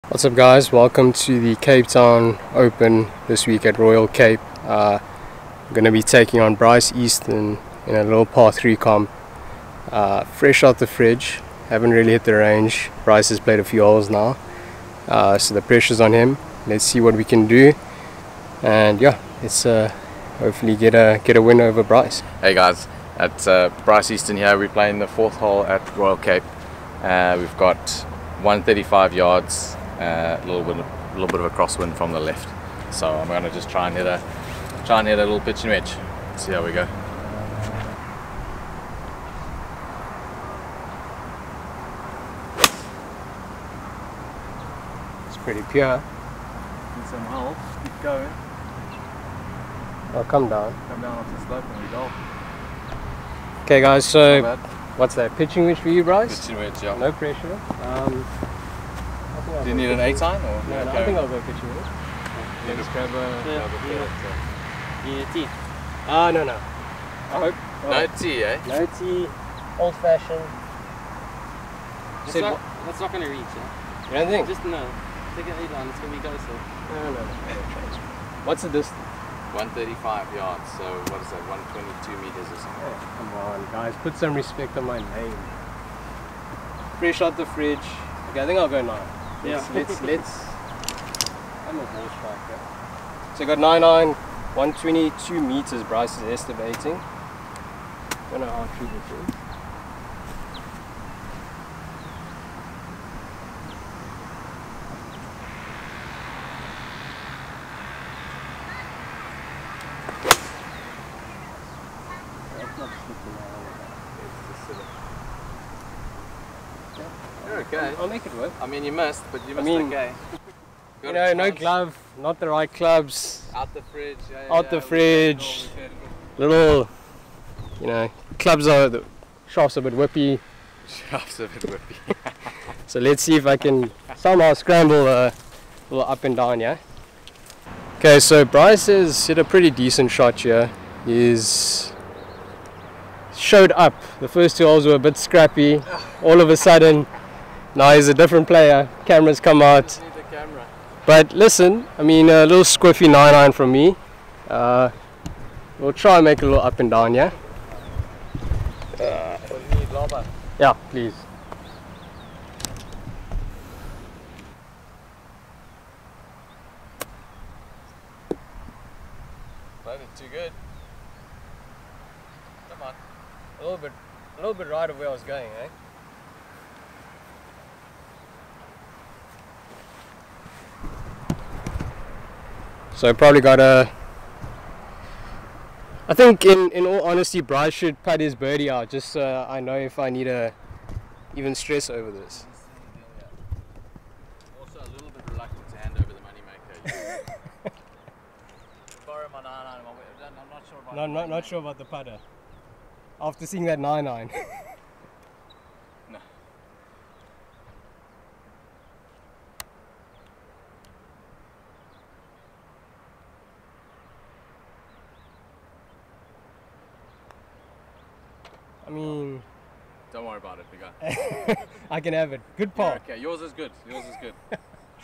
Beat the Pro Challenge ft. Bryce Easton || Pro Golf Vlog
Hi Ladies & Gentleman ! Welcome back to the channel!

Enjoy this par 3 challenge and some banter with the legend Bryce Easton!